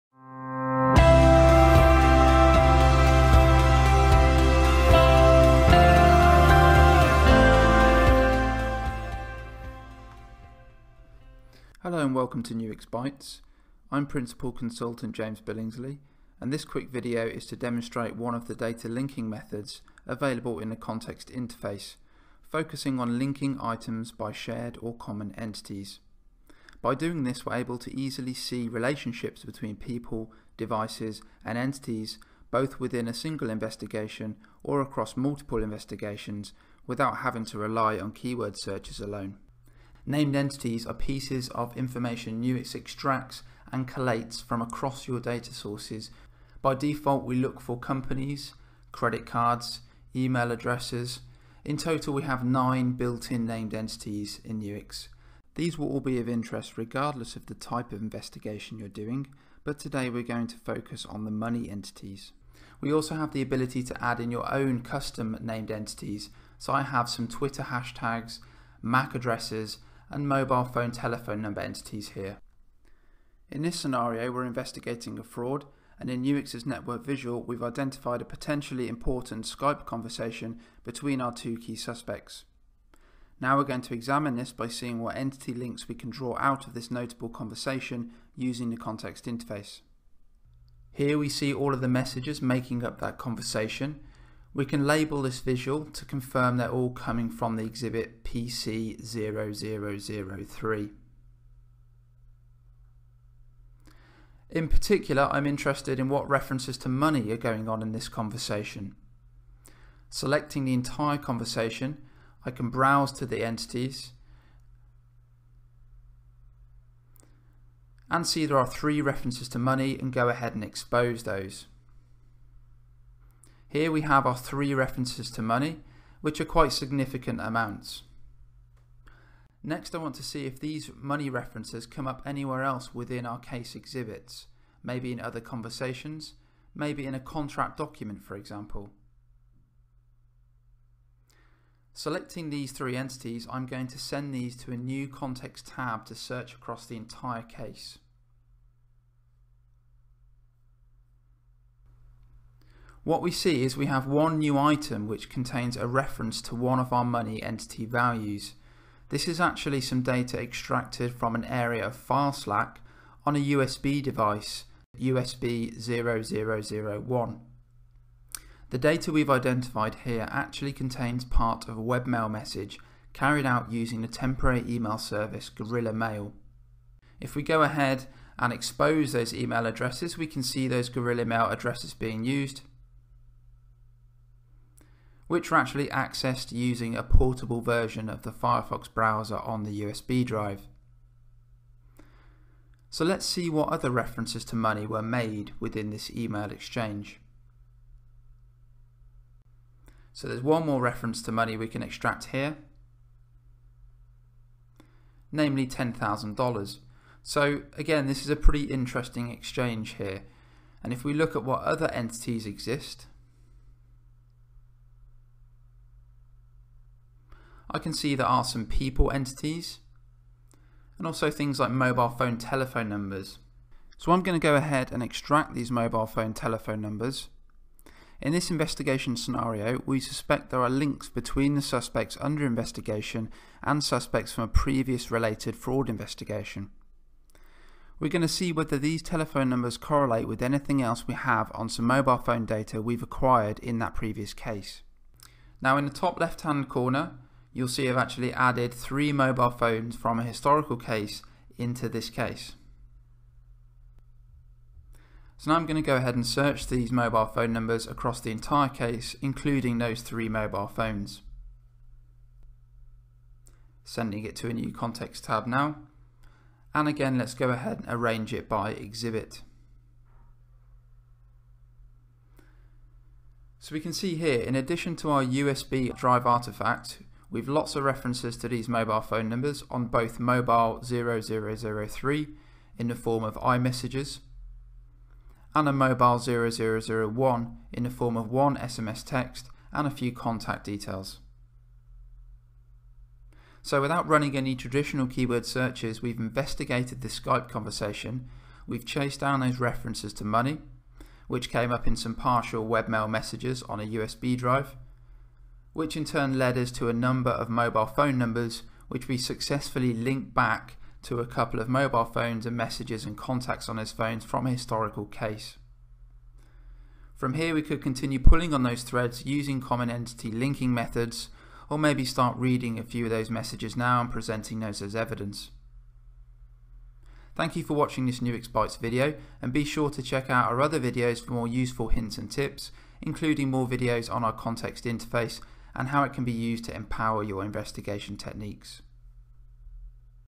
Data Linking in Context – Exposing Entities
This short video shows one of the data linking methods available in the context interface focusing on linking items by shared or common entities.